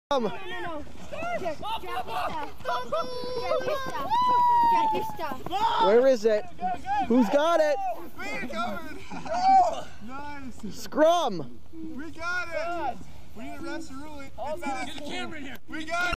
Fumble Scrum
A double team blitz by Kyle Traynor and Jamaal Franklin on QB cause a fumble, and a multiplayer scrum ensues during a 2002 NBSFL game.